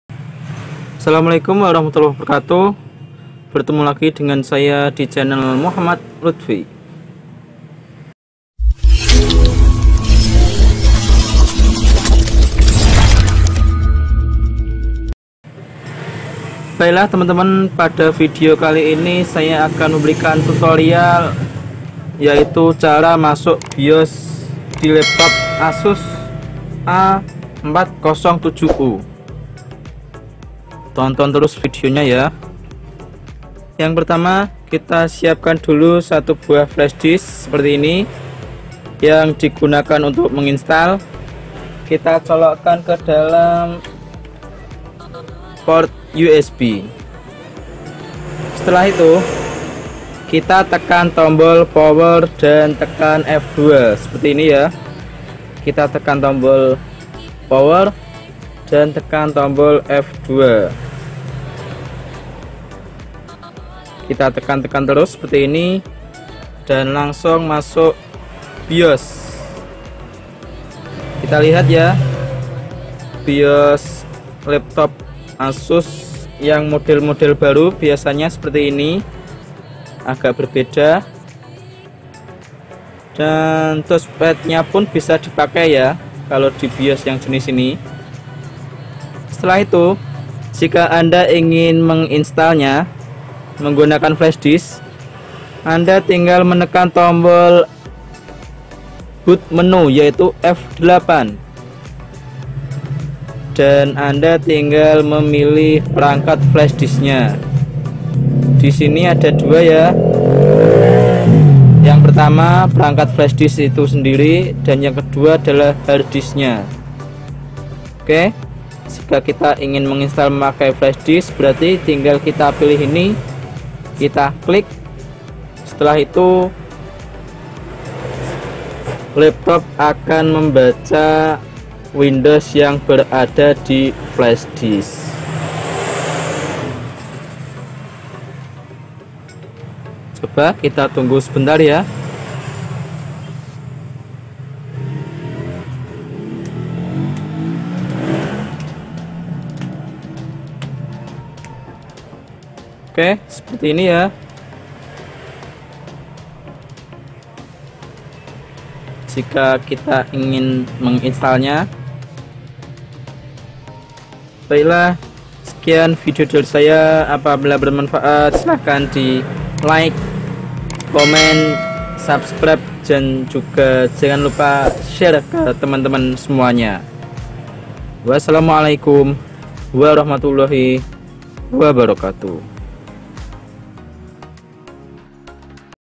CARA MASUK BIOS LAPTOP ASUS A407
Berikut ini tutoral cara masuk bios laptop asus a407, disimak ya gaes biar gaes - gaes bisa melakukanya dengan betul - betul benar.